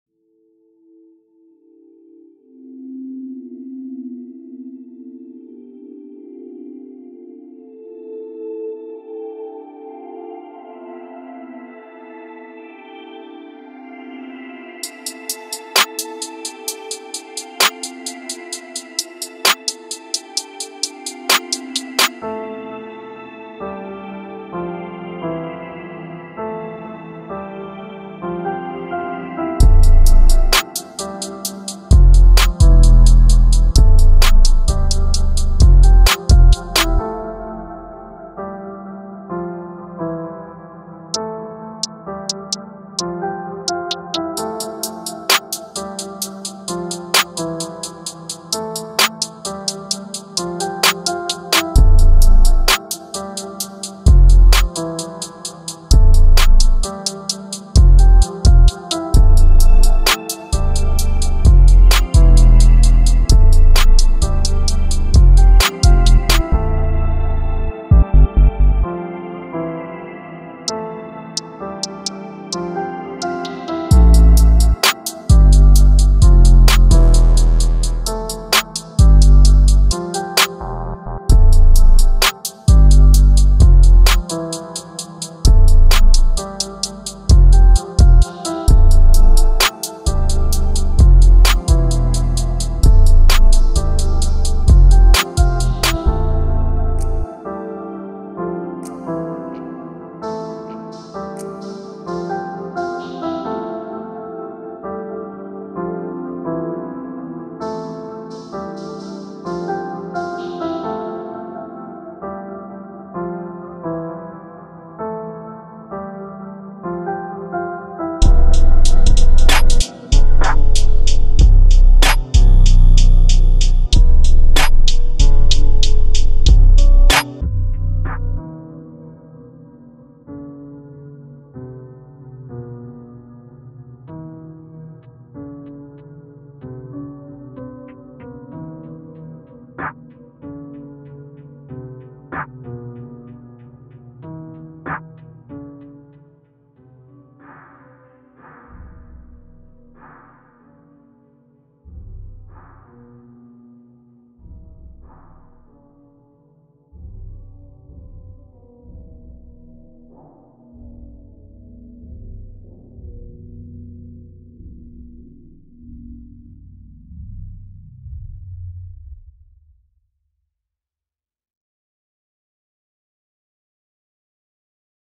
(Free/Untagged) Ginseng X Summrs X Comethazine X Luisss X Night Lovell X XXXTentacion X F1lthy
This is one of the few i can say i made myself fully ! thanks for any support :) crazy stuff rlly soon !
Tempo/BPM is 130 just hmu for any original files / stems / flps / questions / etc
Free to use just credit (prod.XRN / prodXRN) Download / listen here too (better quality) vvv
If anyone makes anything also let me know please I'd love to see any work / Collab
timestamps-
00:00 hi
01:57 slowed ending / outro